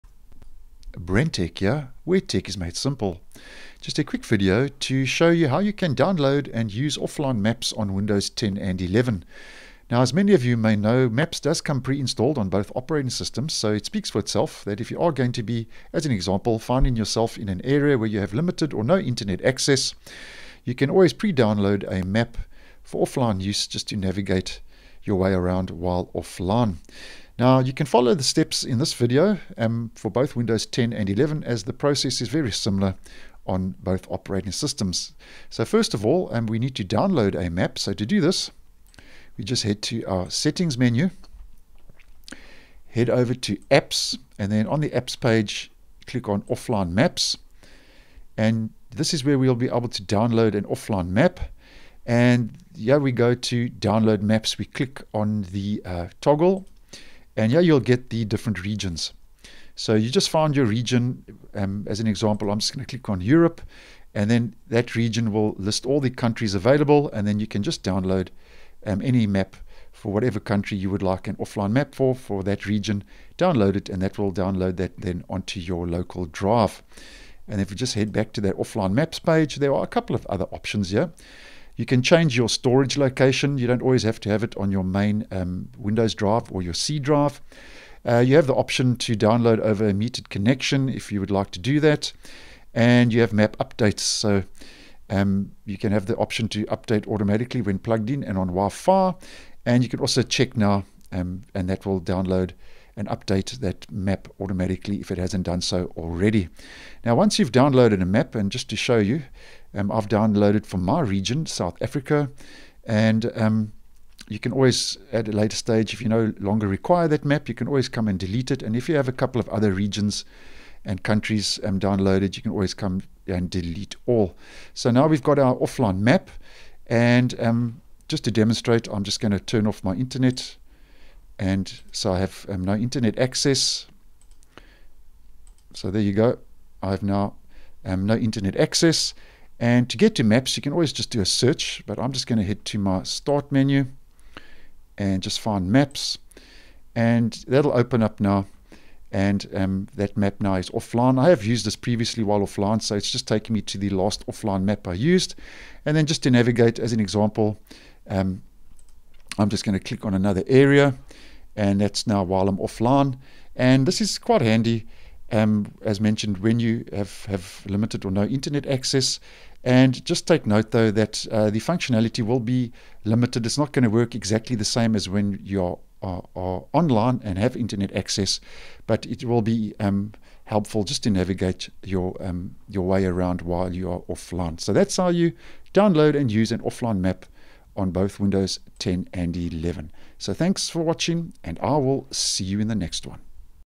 How to download and use Offline maps on Windows 10 and Windows 11
If you are going to be in an area where you will have limited or no internet access, downloading a map for offline use can be handy and help you navigate your way around while offline.